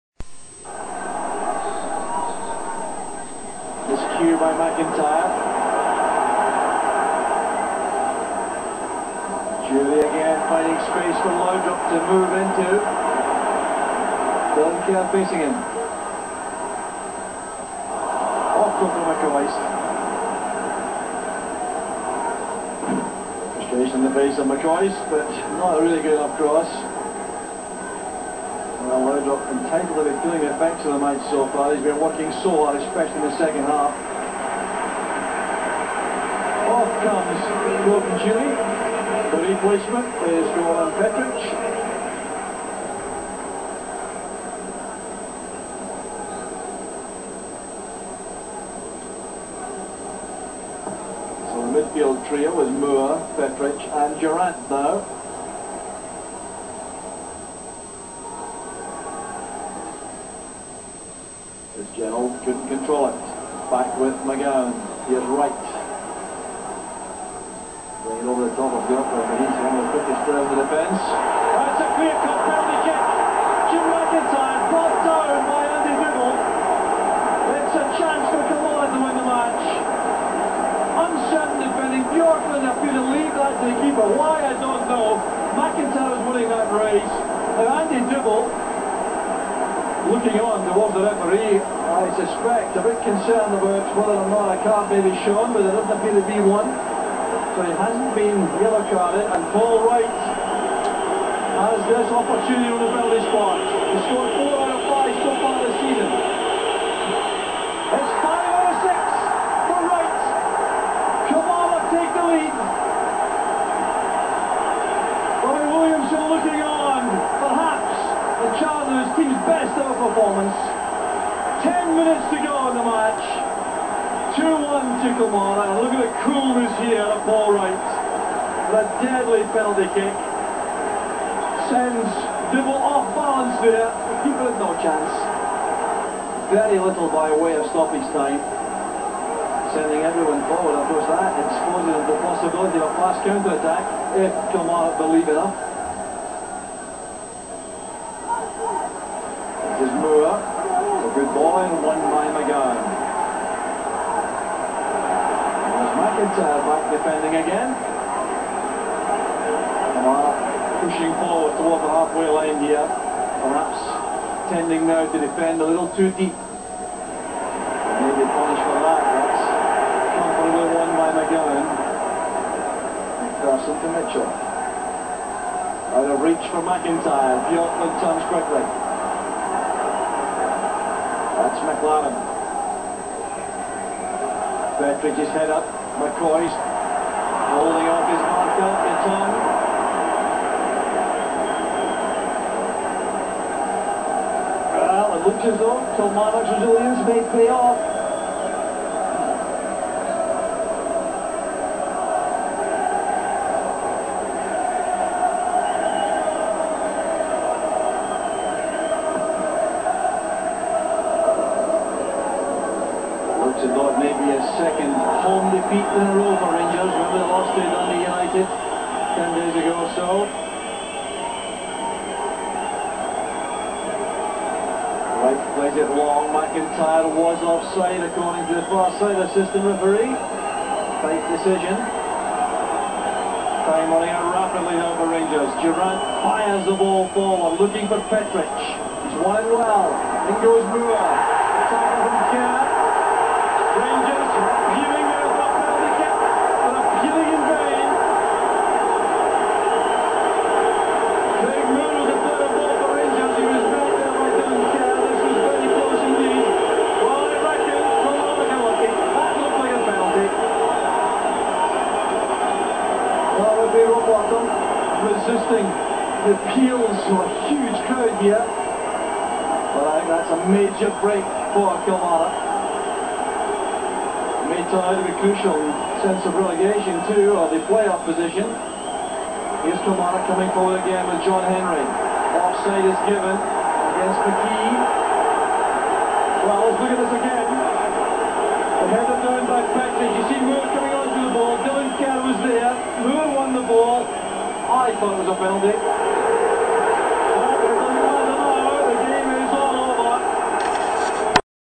Rangers 1 2 Kilmarnock (Mar 22/97) PART 3
Killie win an important match at Ibrox.